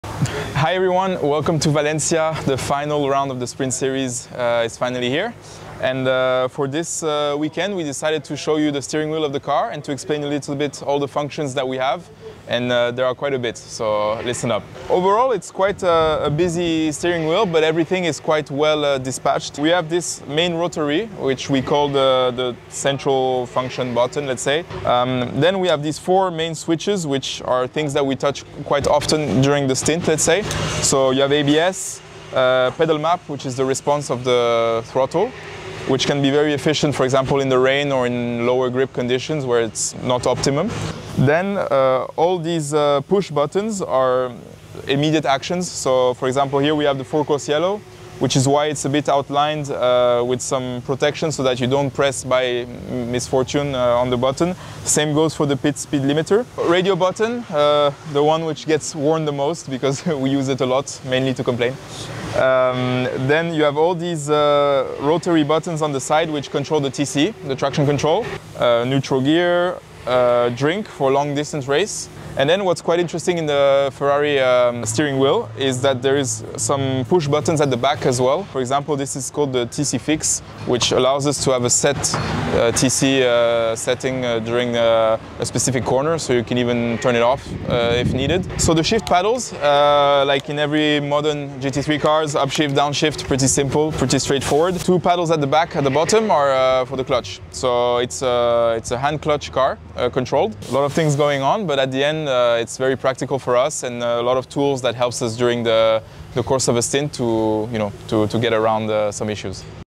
How the Ferrari 296 GT3 Steering Wheel Works (Explained by Vincent Abril)
How does the Ferrari 296 GT3 steering wheel work?
Vince Abril takes you Behind the Yellow Stripe and breaks down the key functions in just a few seconds.